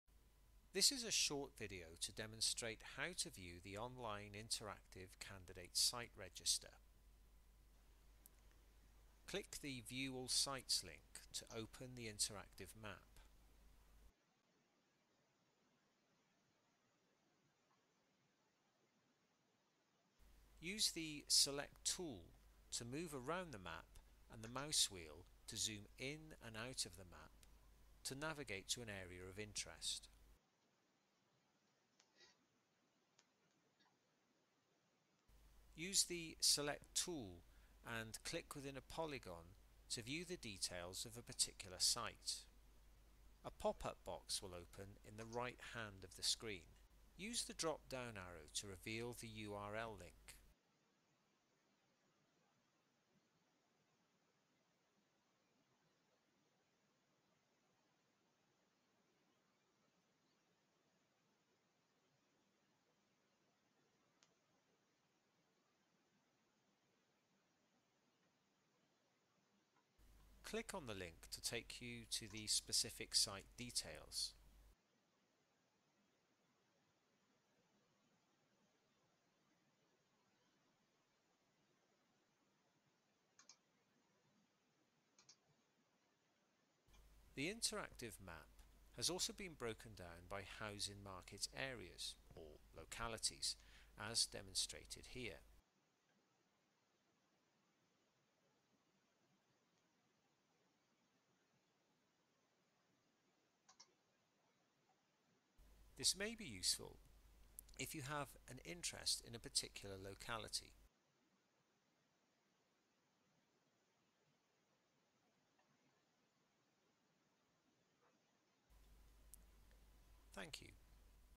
Replacement Local Development Plan (2022 -2037) - Viewing the interactive Candidate Site Register
This is a short video to demonstrate how to view the online interactive Candidate Site Register.